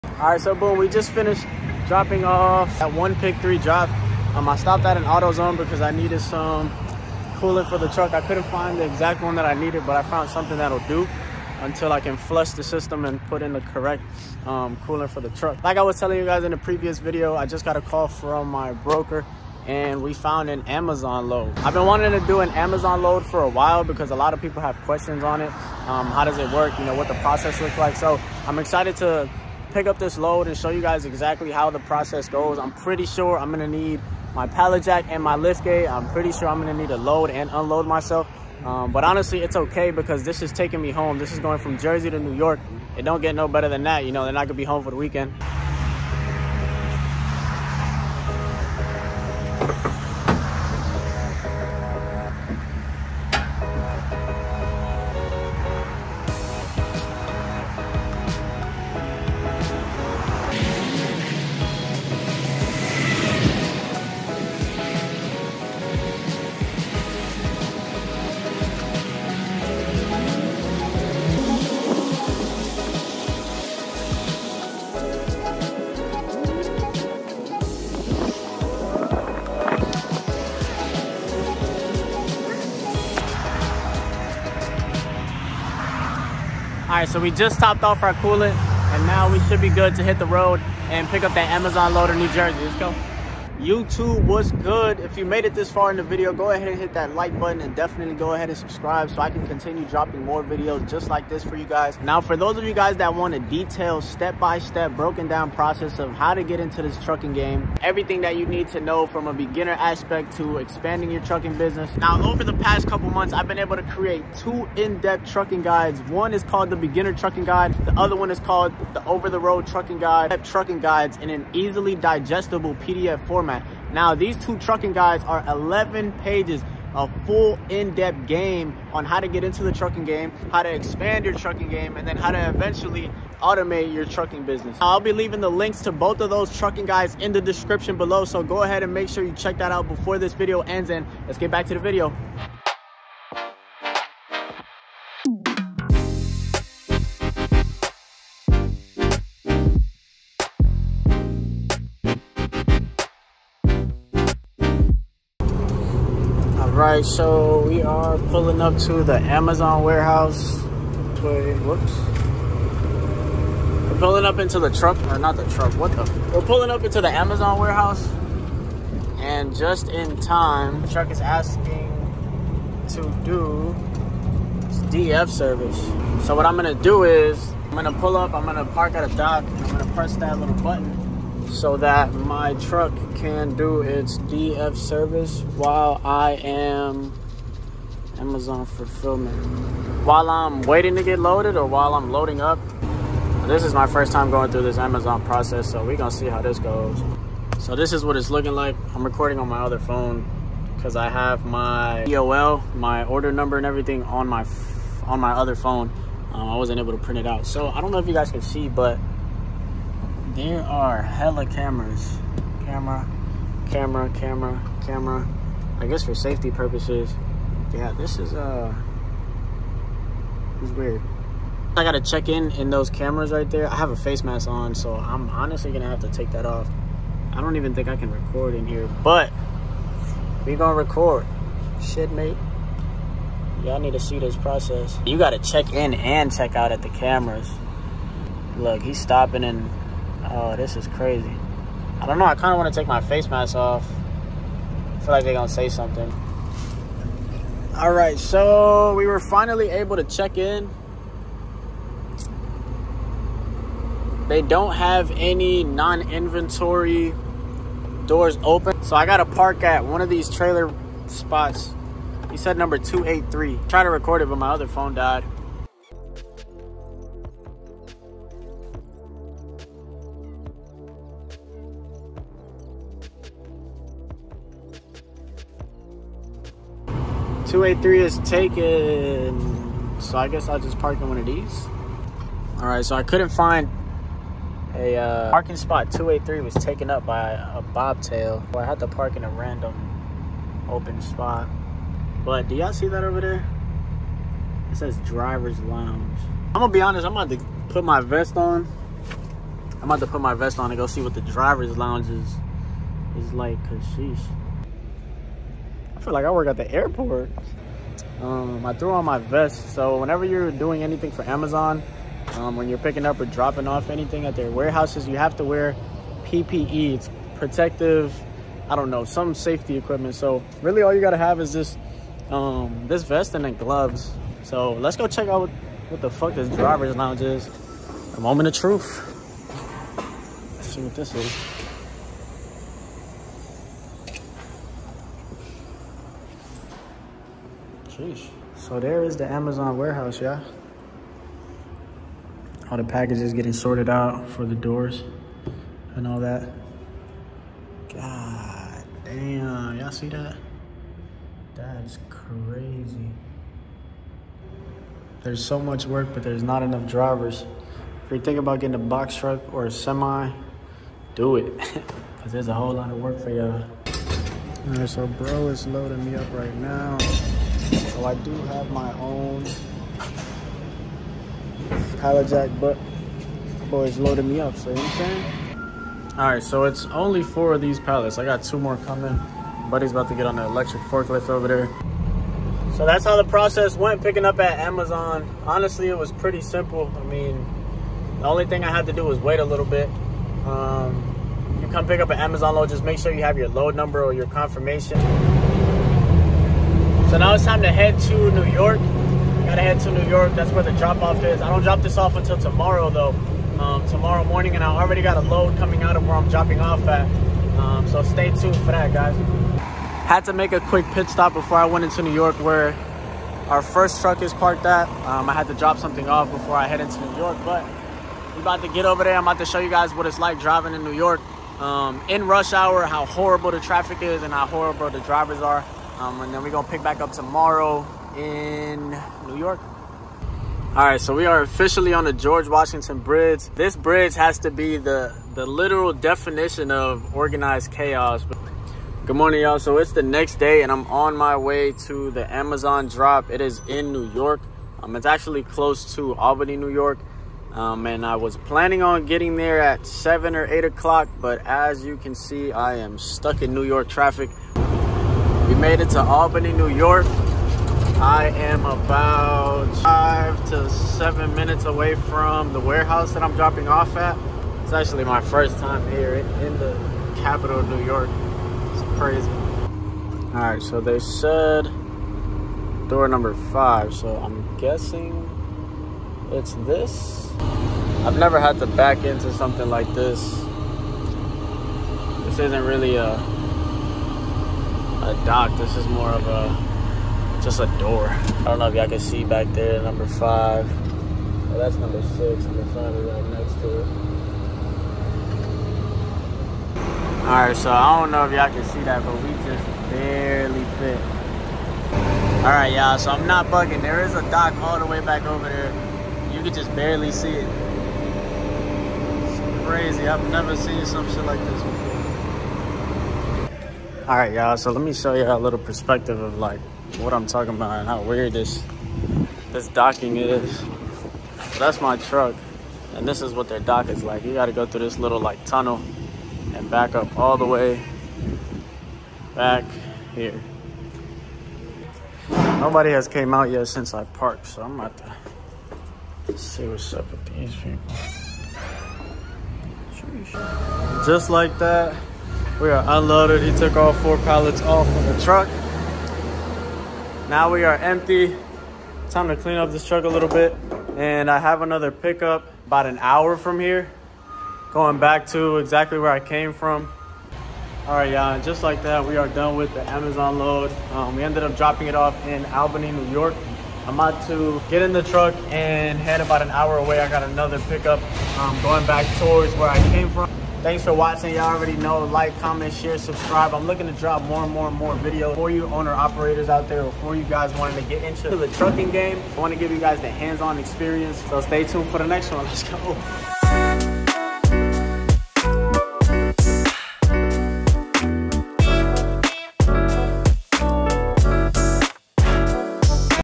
Box Truck Business New York 2022 | First Amazon Load !!
Amazon load to get me home from New Jersey to New York

Amazon Trucking guide coming soon !!